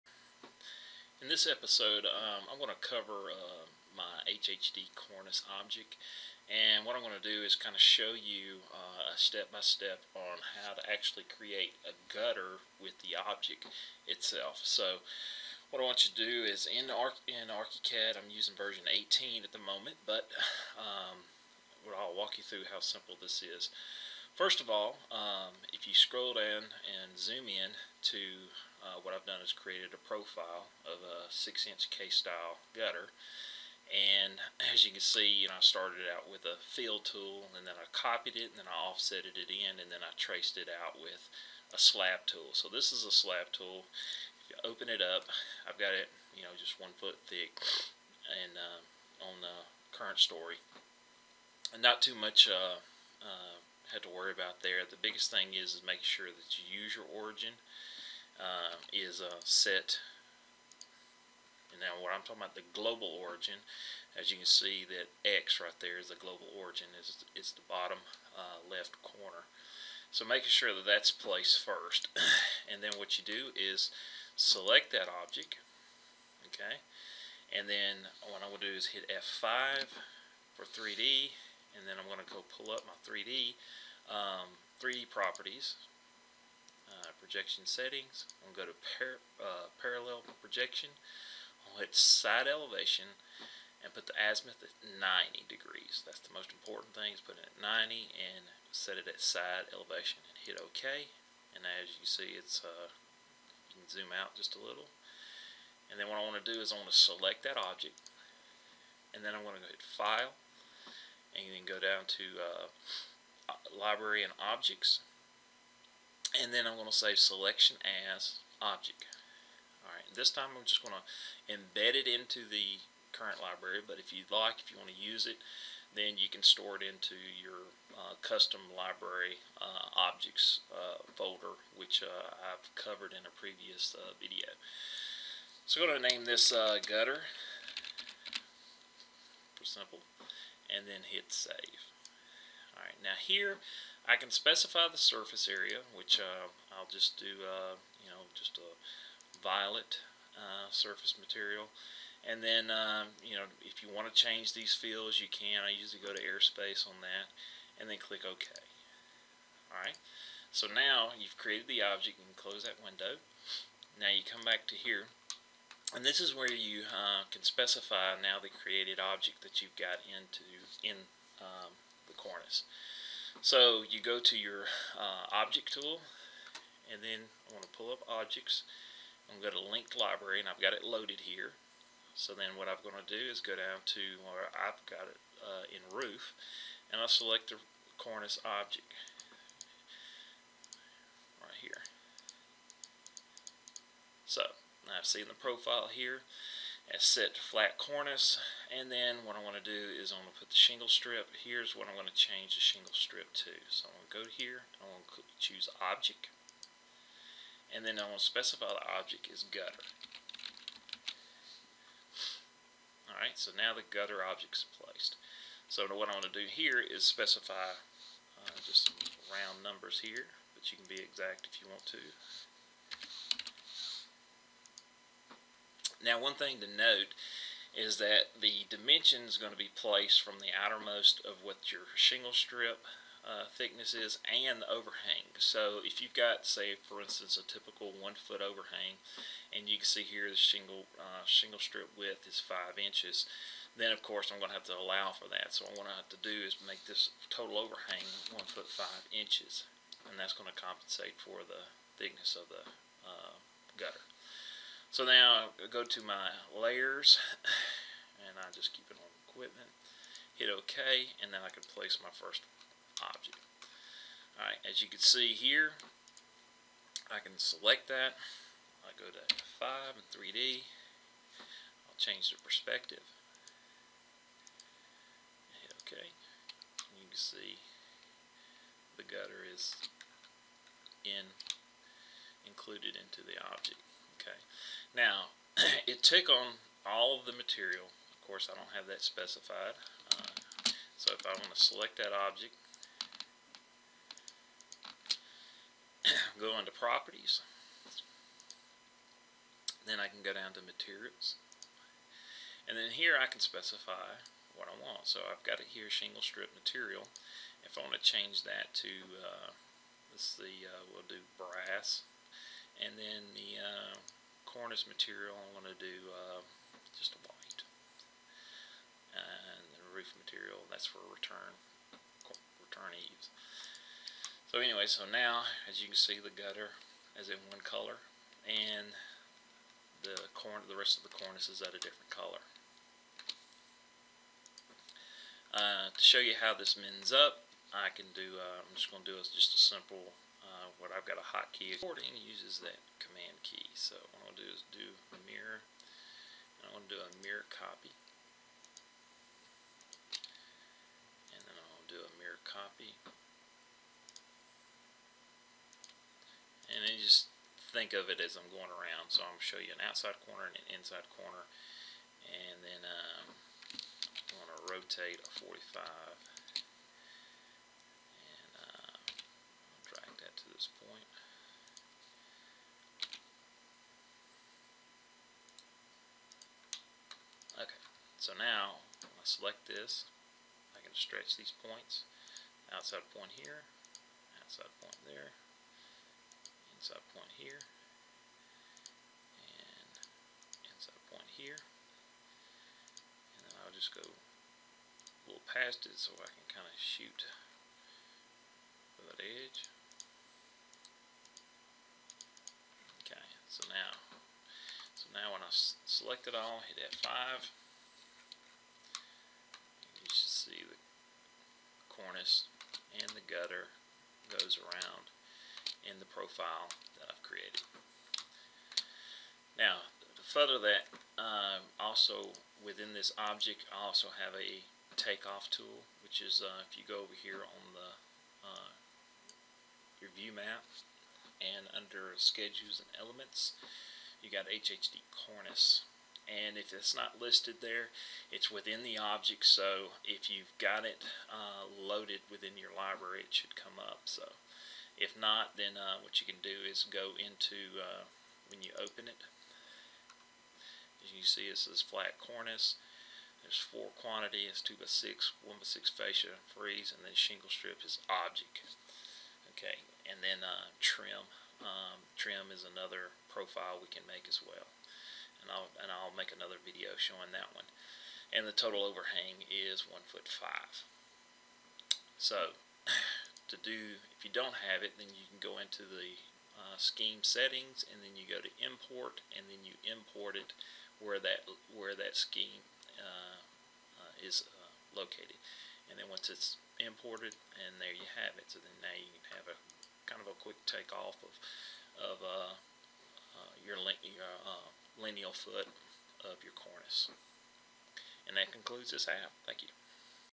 HHD-Cornice Object Adding a gutter using ArchiCad
This video will show you how to add a gutter profile within the HHD-Cornice object used with ArchiCad